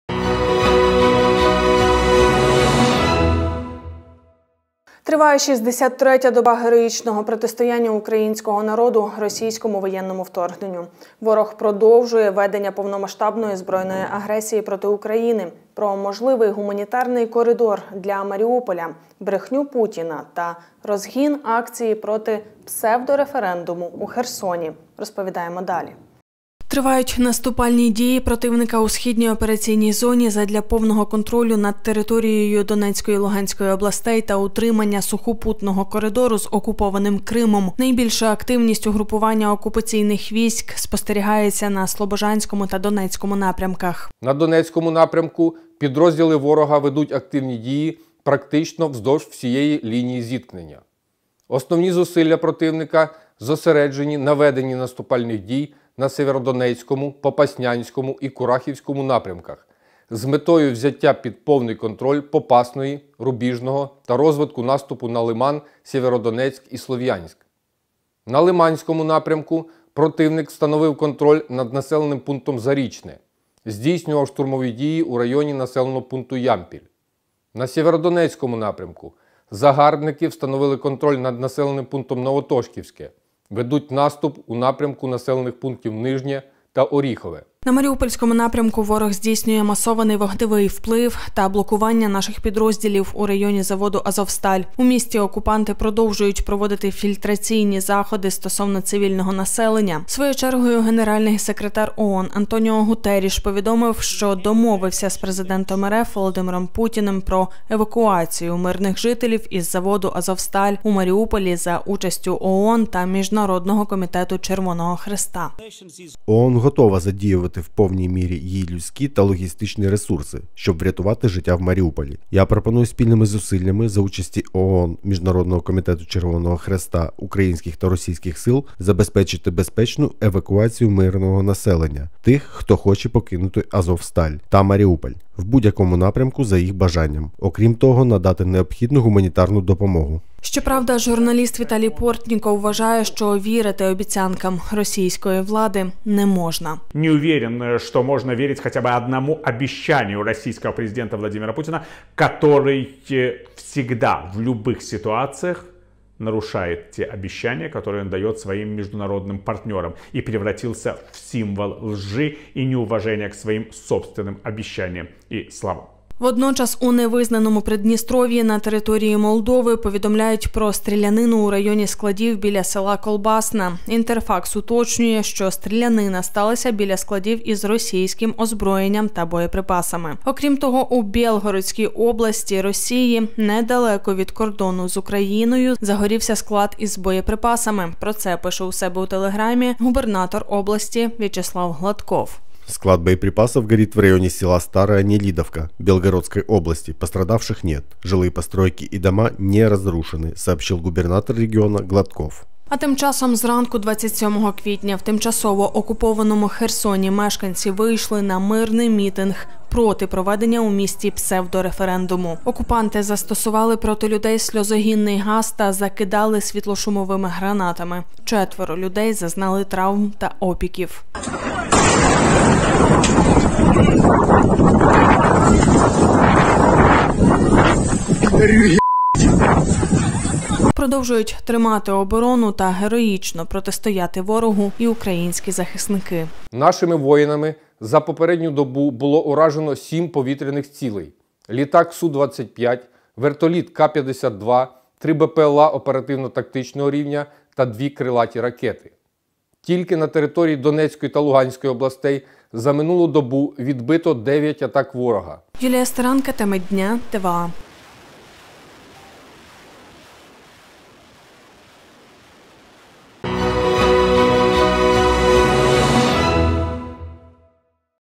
Війна - 63 день: брехня путіна, вибухи в росії та розгін акції проти псевдореферендуму у Херсоні
Триває шістдесят третя доба героїчного протистояння Українського народу російському воєнному вторгненню. Ворог продовжує ведення повномасштабної збройної агресії проти України.